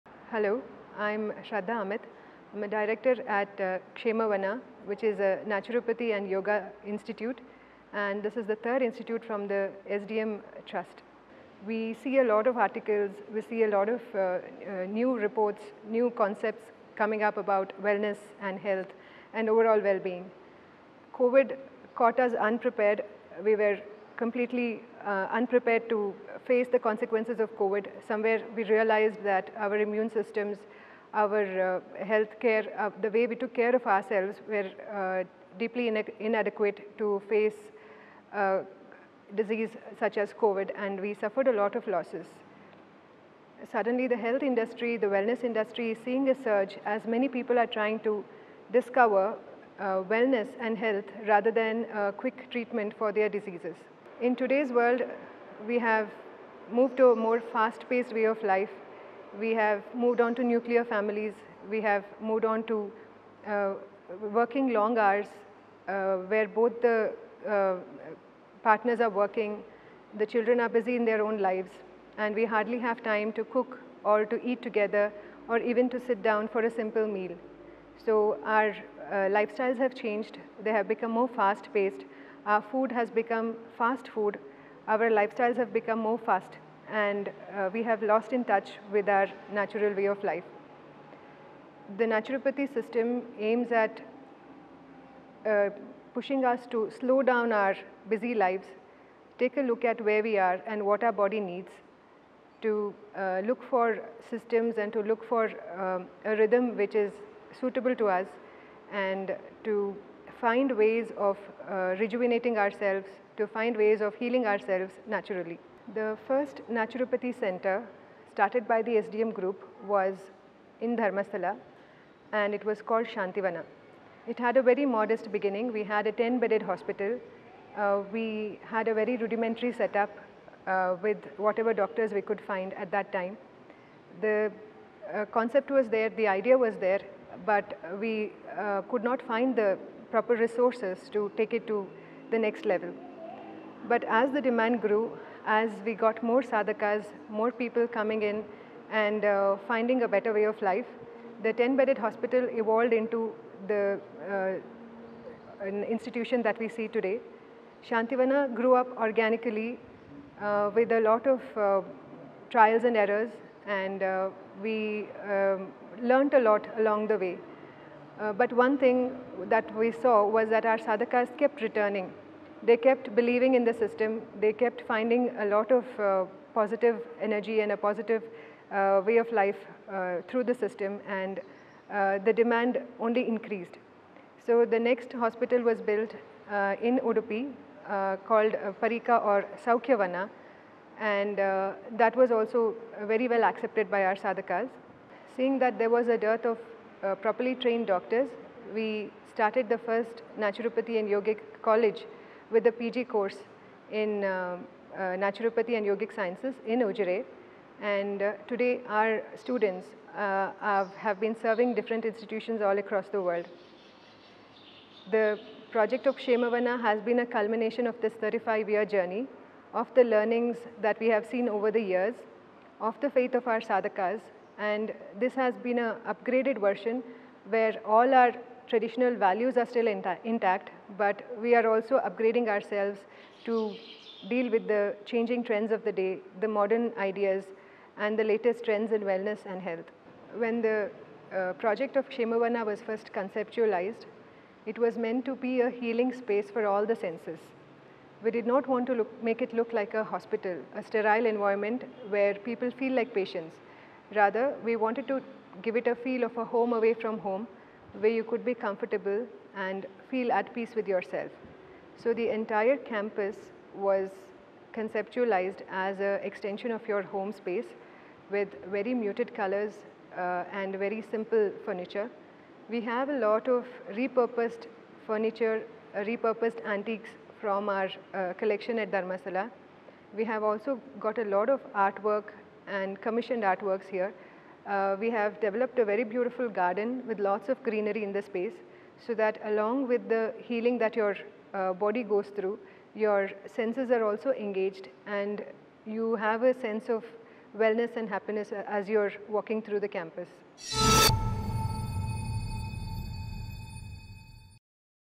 Pills are passe; people today are exploring alternate ways of healing: Shraddha Amith, Kshemavana
Shahid Akhter, editor, ETHealthworld, spoke to Shraddha Amith, Director, Kshemavana, SDM Institute of Naturopathy and Yogic Sciences, to figure out the growing awareness and demand for slow-paced, natural healing that tends to fix the rhythm disrupted by a hectic lifestyle.